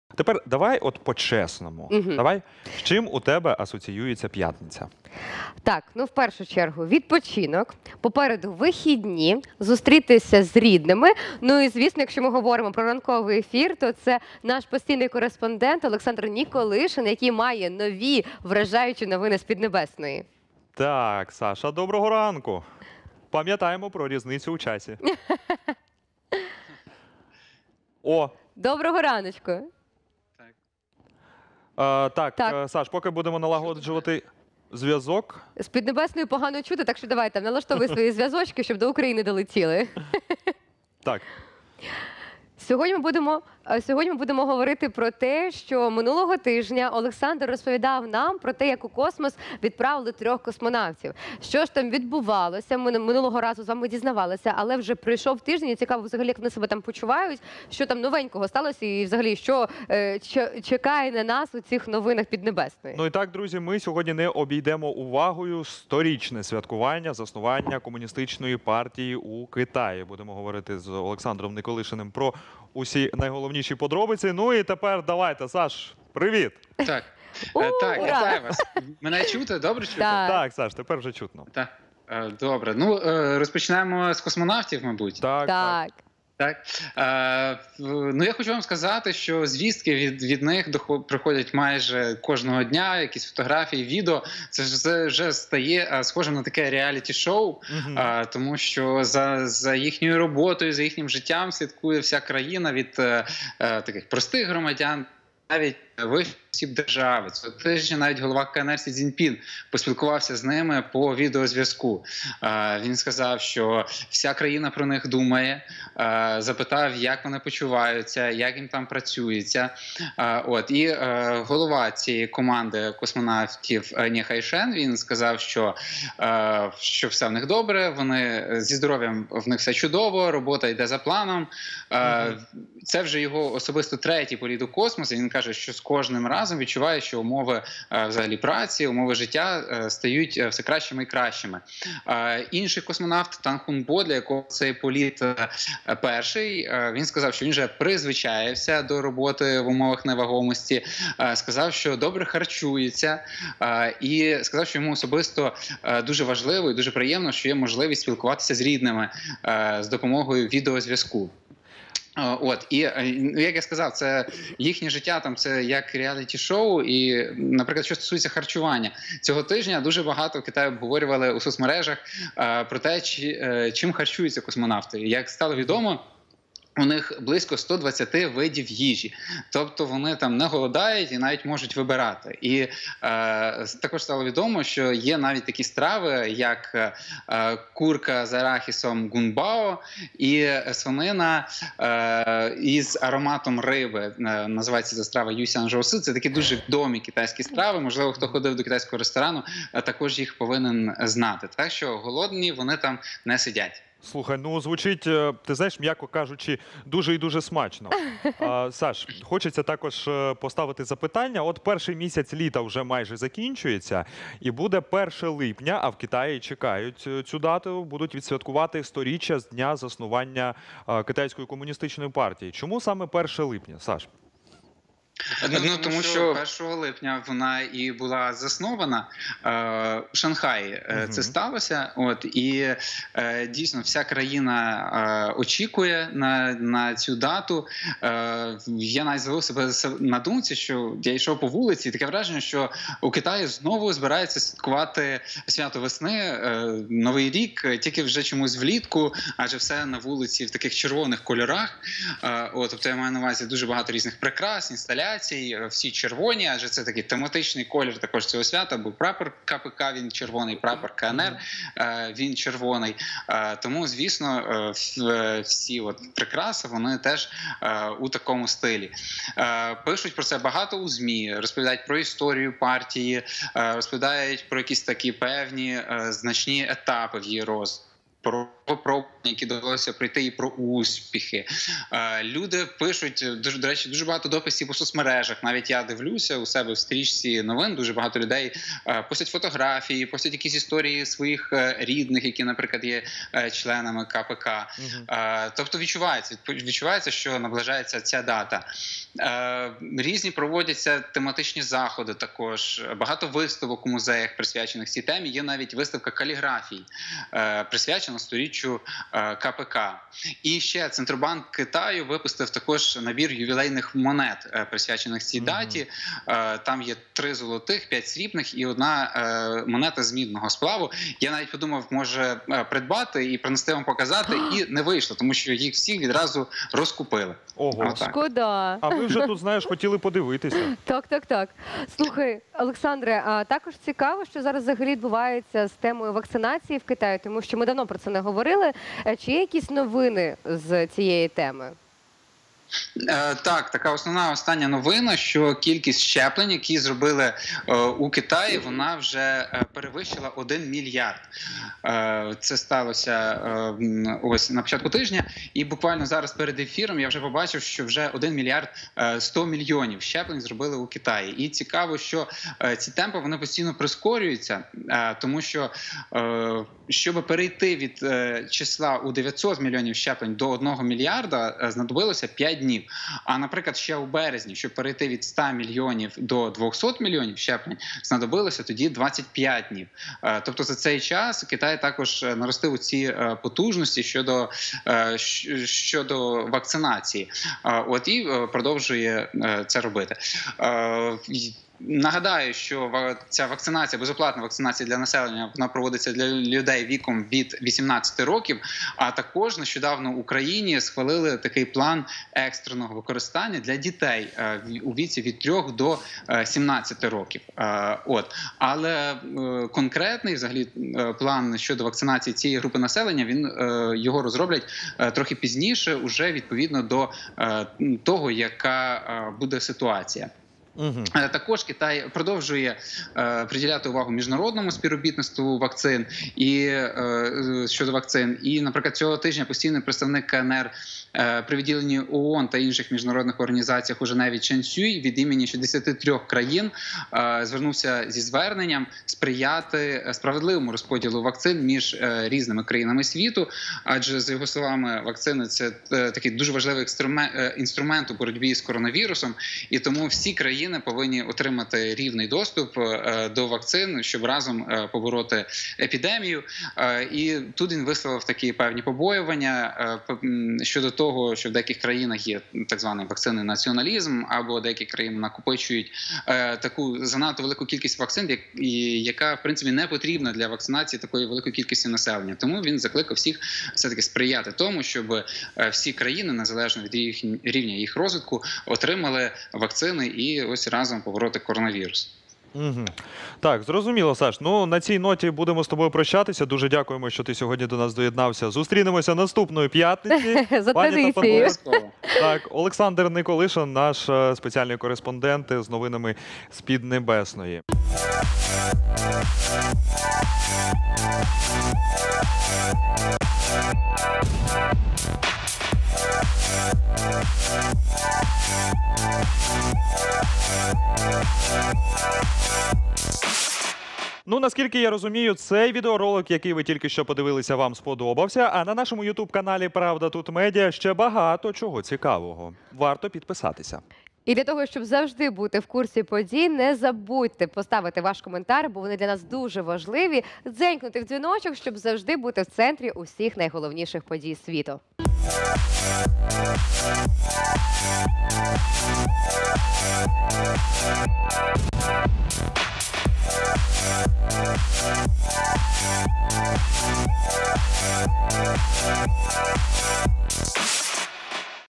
ПравдаТУТ LIVE: Олександр Николишин про актуальні новини з Піднебесної
Не можемо не розказати про актуальні новини прямісінько з Піднебесної з нашим кореспондентом Олександром Николишиним. Одразу розпочнемо про те, що минулого тижня Китай відправив на орбіту астронавтів, як вони себе почувають та чому вони не повернувшись на Землю з радість полетять до космосу знову. Також 1 липня в Китаї будуть святкувати 100 річчя утворення Комуністичної Партії Китаю, і як проходить підготовка до святкування. І грандіозне розчарування – яку монету не встиг купити Олександр Николишин. Також в інтерв’ю останні новини з приводу вакцинації від ковіду дивіться в нашому інтерв’ю.